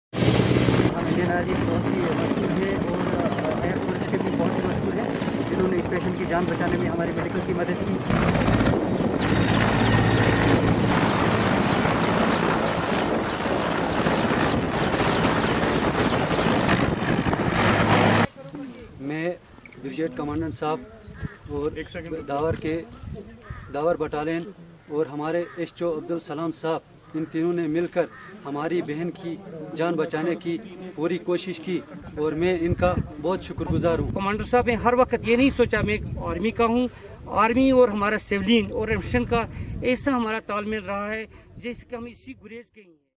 SNOW LEOPARD BRIGADE EVACUATES PREGNANT LADY STRANDED IN SNOW CLAD GUREZ* *ABD NEWS 24×7* Daw
*SNOW LEOPARD BRIGADE EVACUATES PREGNANT LADY STRANDED IN SNOW CLAD GUREZ*

*ABD NEWS 24×7*

Dawar Army Camp of Snow Leopard Brigade airlifted a pregnant lady stranded at Dawar for last four days. Presently, the Gurez Valley is cut off from the rest of the Kashmir Valley due to high snow levels at Razdan Pass. The prevailing bad weather conditions for last four days did not afford any window of opportunity to the Army to evacuate the patient by air.

 On 01 Apr 20, Dawar Army Camp of Snow Leopard Brigade received a requisition from civil administration for evacuation of Zytoona Begum, resident of Satni Village located very close to the Line of Control who had been critically ill. She was a case of Molar Pregnancy with excessive bleeding resulting in low Haemoglobin and needed urgent surgery. She was under the close monitoring of civil and Army medical team at the Hospital at Dawar. Finally, the patient was evacuated from Army Helipad in Gurez at the first available opportunity late afternoon today .

 The threat of COVID-19 further added to the procedures of evacuation. Dawar has a civil hospital with limited advance diagnostic and treatment facilities. In these difficult times of the Corona pandemic, the medical team of Dawar Army Camp and Sub District Hospital in Gurez has achieved a high degree of cohesion and synergy with an aim to provide medical aid to locals and Army personnel alike. With the pooled resources of Army and Civil Administration, a quarantine facility has been created at Dawar in which five civilians who had recently returned from Bandipora have been quarantined.

  Soldiers of Snow Leopard Brigade activated the Gurez Helipad at short notice and the critically ill patient was continuously monitored before she could be evacuated by helicopter. She is presently undergoing treatment at Lal Ded Hospital at Srinagar.